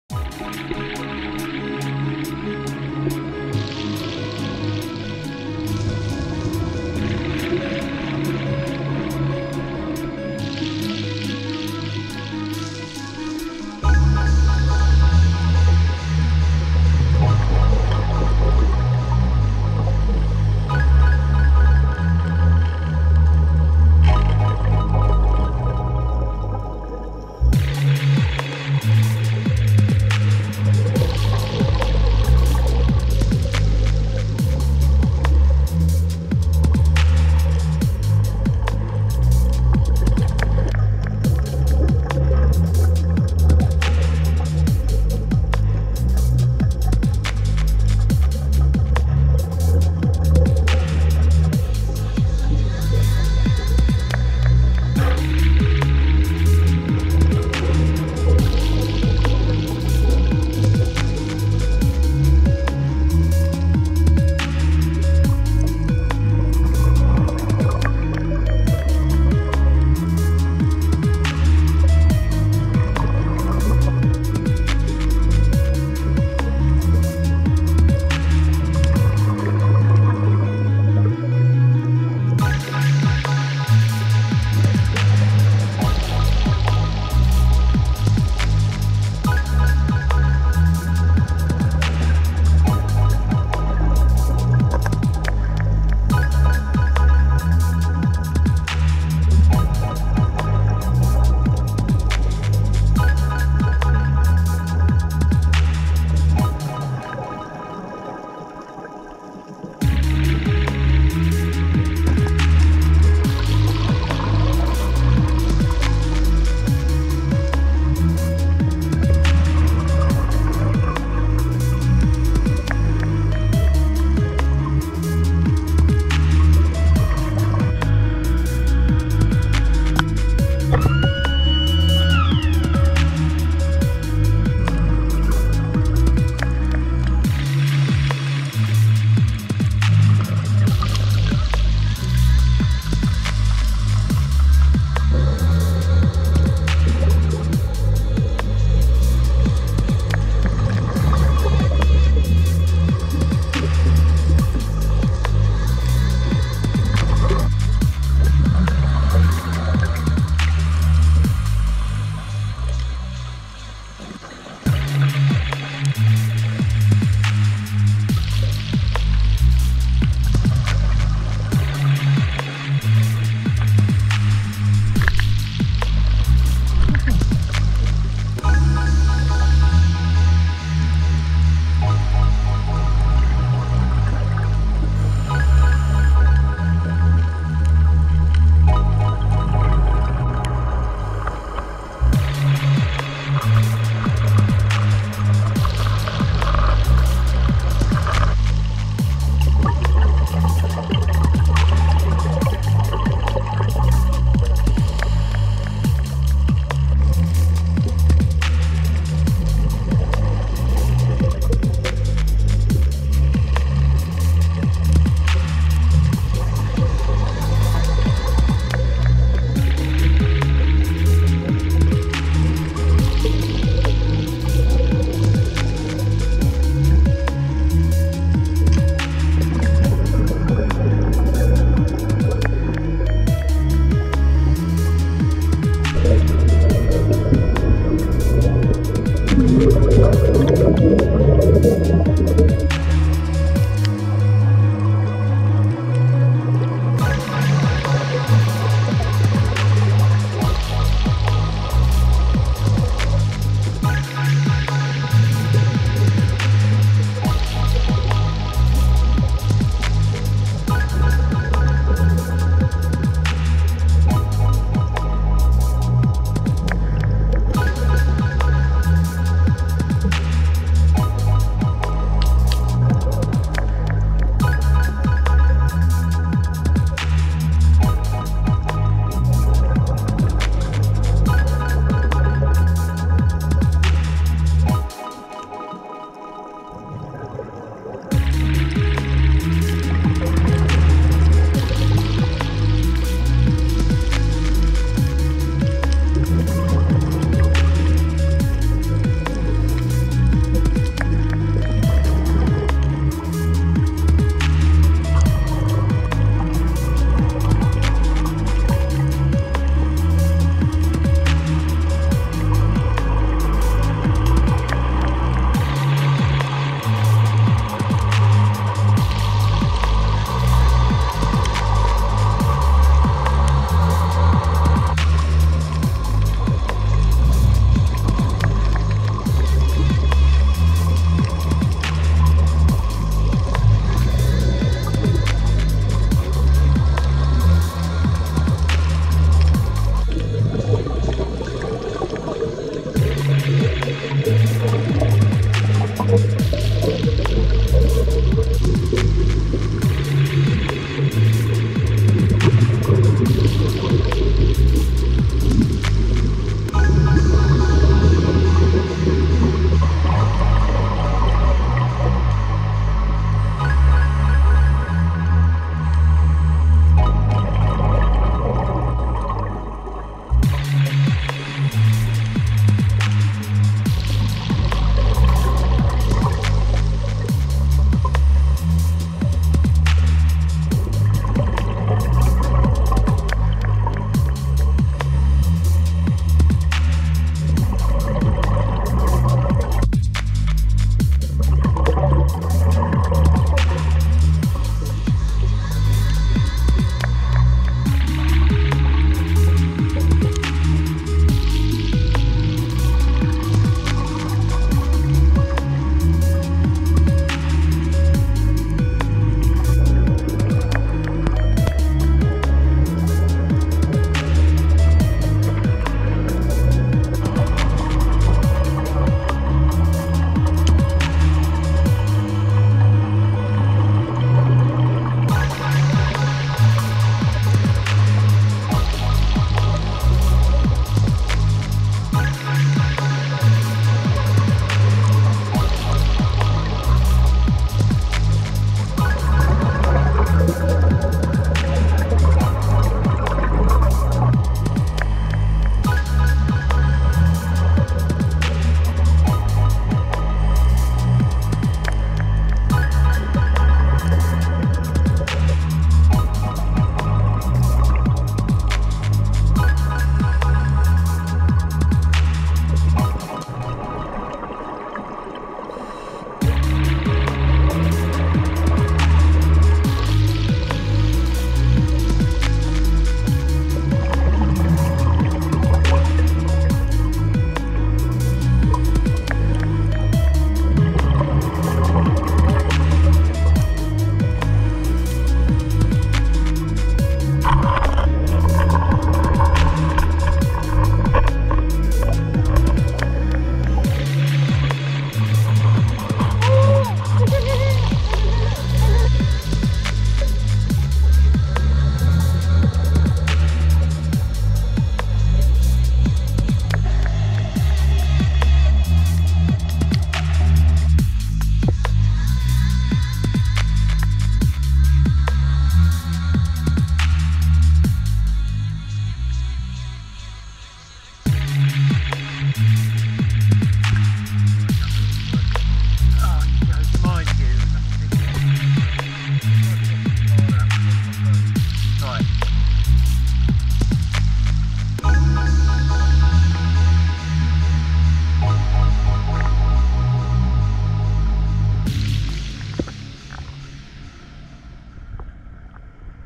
Diving The Similan Islands During Covid-19 😷🤿
Our trip on M/V Sawasdee Fasai Feb. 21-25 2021 at The Similan Islands & Richelieu Rock.
Once again, huge thanks 🙏🏻 to West Coast Divers for another awesome trip.

👍 LIKE the vid Thumbs Up Button
In this video you can see the perfect diving conditions, with clear-blue water and healthy marine life. There were very few other boats in site and the weather was perfect!